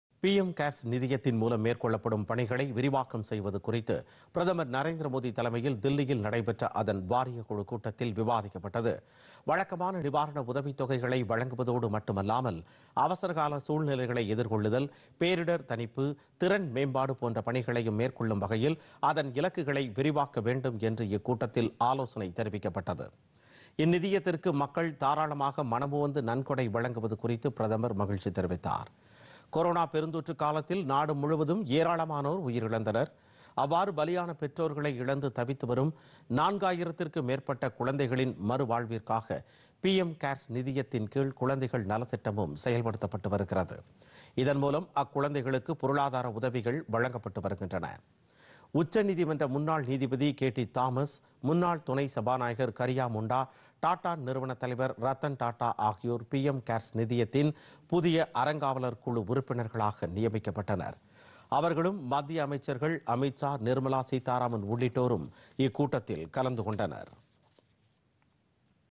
பிஎம் கேர்ஸ் - முக்கிய ஆலோசனை
பிஎம் கேர்ஸ் நிதியத்தின் செயல்பாடுகளை விரிவாக்கம் செய்வது குறித்த நிர்வாகக்குழு கூட்டம் - பிரதமர் நரேந்திர மோதி தலைமையில் முக்கிய ஆலோசனை.